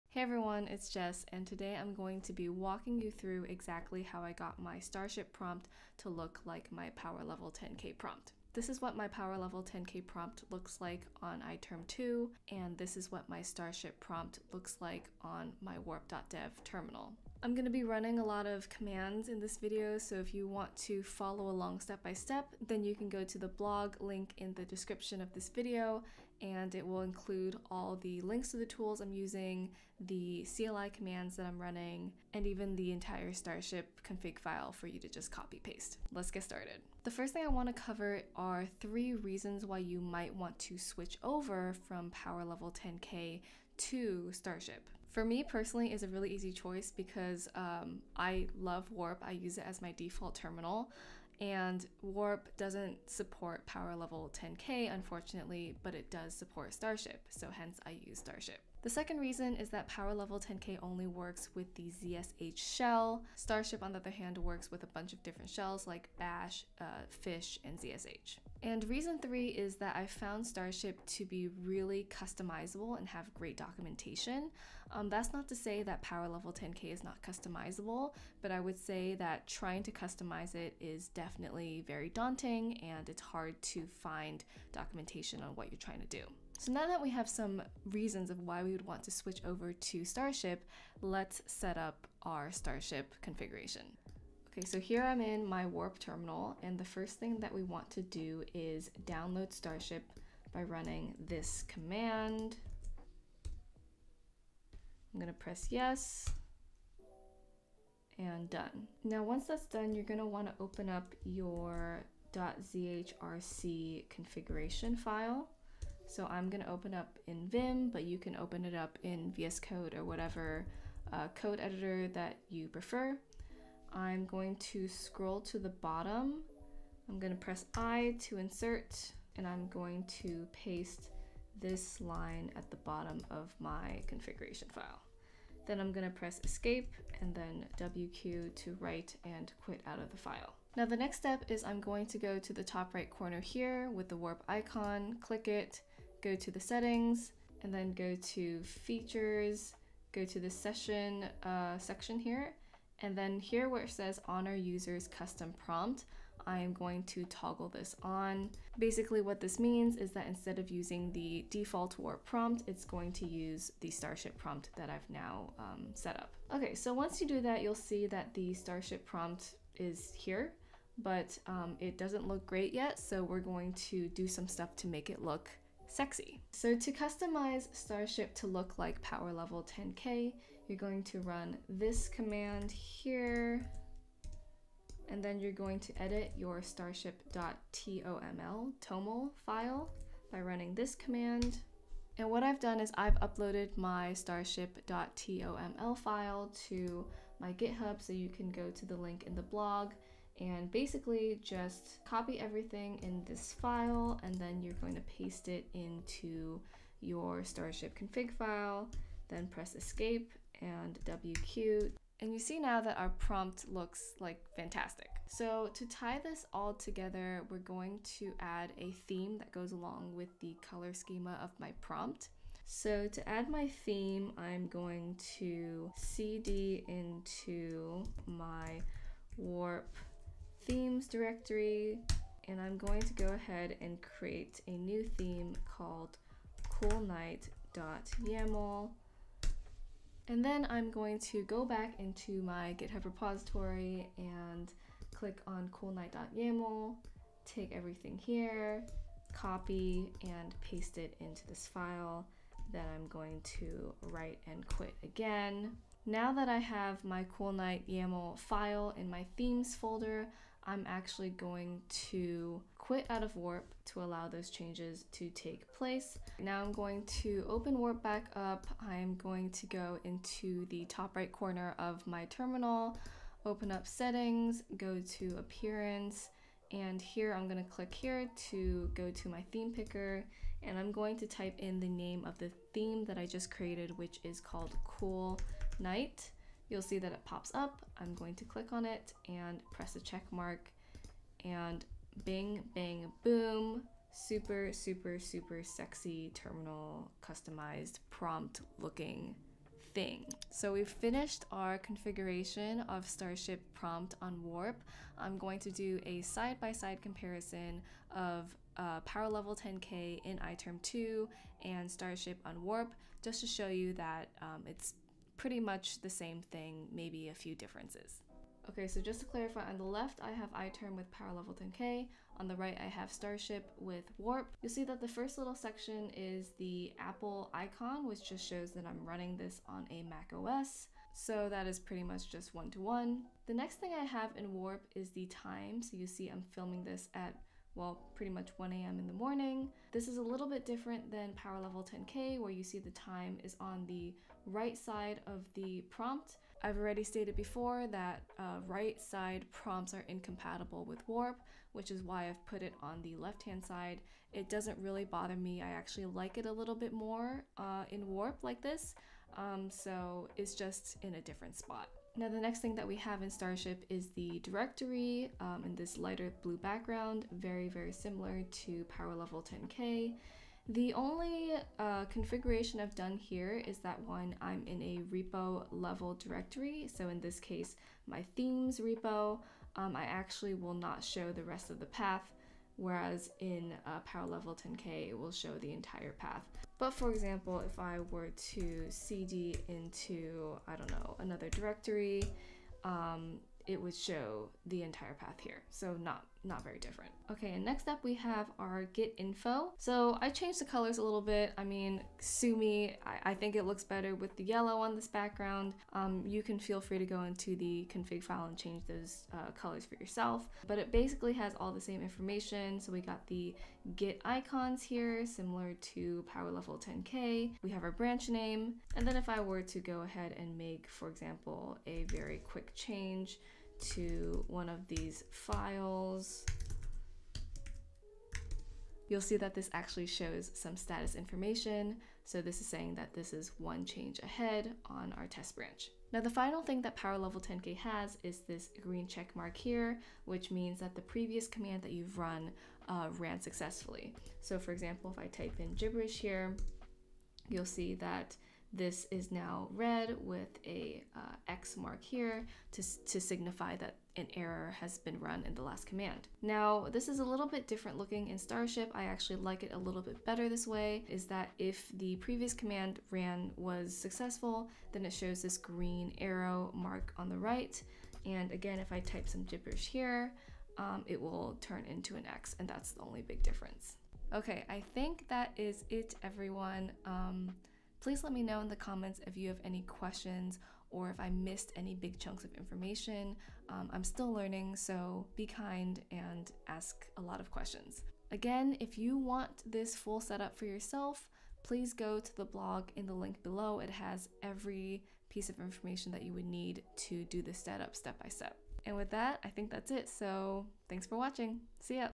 How to Configure Starship to Look Exactly Like P10K (zsh + Warp + MacOS)
In this video I'll be sharing exactly how I setup my Starship prompt so that it looks and feels exactly like powerlevel10k. I'm not sure why there's so little resources on the Internet around this, but hope this helps anybody else who's wondering how to do this :-)

⏰ Timestamps
00:00 Introduction
00:37 Why would you want to switch to Starship prompt?
01:34 Setting up our Starship configuration
03:35 Downloading a theme to tie the colors together
05:01 Side-by-side comparison of p10k to Starship
07:55 Conclusion